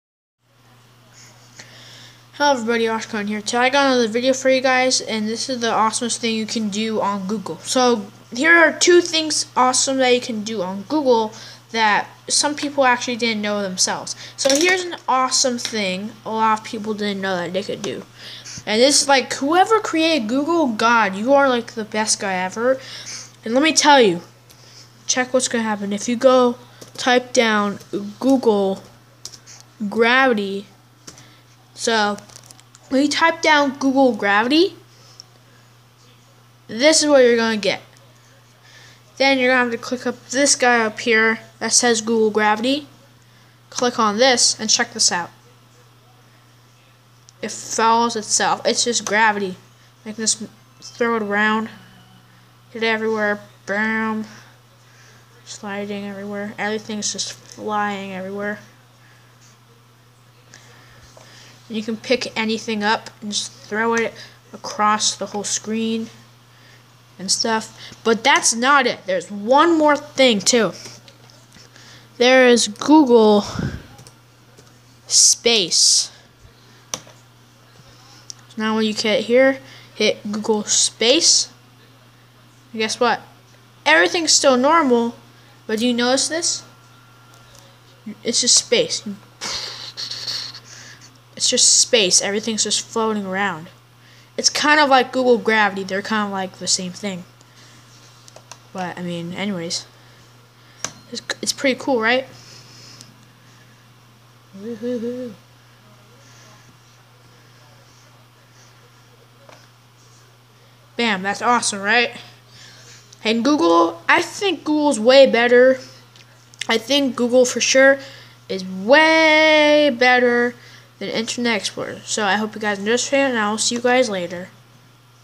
2 amazing things that you can do on google
1 of the best thing on google is GOOGLE GRAVITY & GOOGLE SPACE and you guys should search this up the it Wil come to a Google page but wait for 3 seconds and then there you have it GOOGLE GRAVITY & GOOGLE SPACE!!!